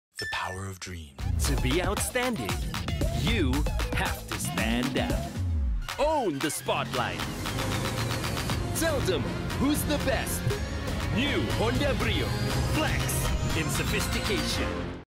New Honda Brio | Flex in Sophistication
Show your flex in sophisticated way with New Honda Brio!

Hadir semakin stand out dengan desain mobil yang lebih sporty serta penambahan berbagai fitur baru yang mempermudah segala aktivitas, New Honda Brio juga hadir dengan berbagai pilihan warna baru.

Tingkatkan kepercayaan dirimu bersama New Honda Brio!